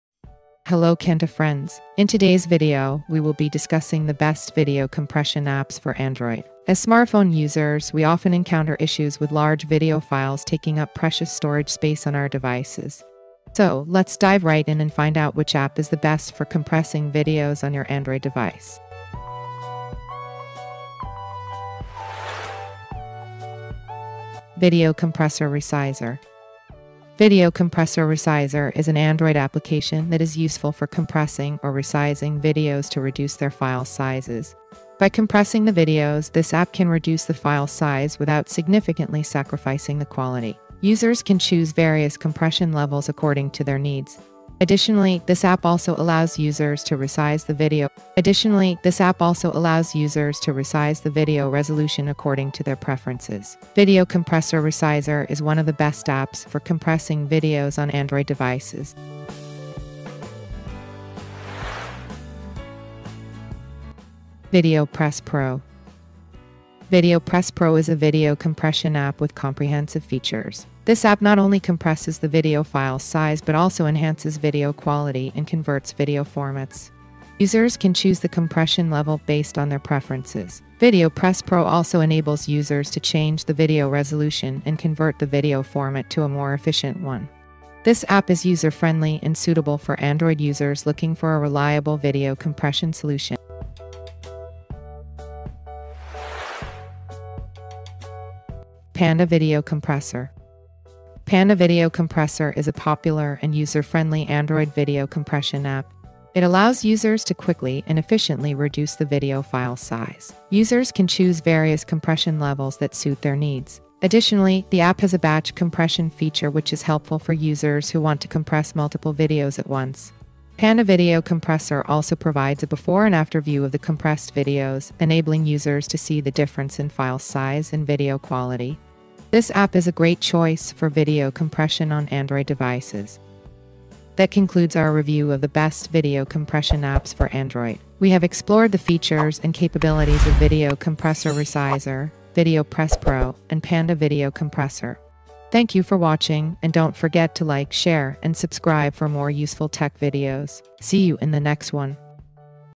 3 Best Video Compressor Apps For Android
Best Video Compressor Apps For Android are applications that can reduce the file size of videos on Android devices. These apps use various compression techniques to compress video files, resulting in smaller file sizes without compromising much on the video quality. These apps are useful for users who want to save storage space on their devices or share videos with others more easily. They are also handy for uploading videos to social media platforms that have file size restrictions.